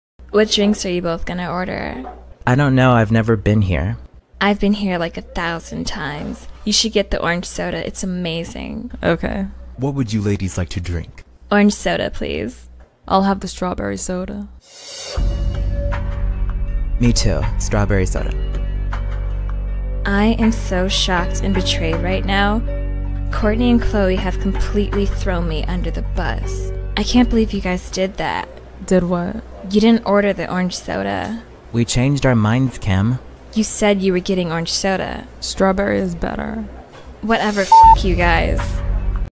Soda Drama (coloured animatic?)
i was bored and this was rushed so-
Ocs: Bombey, Sheila, Raela (light blue thing), Stank (blue cat) and Bobo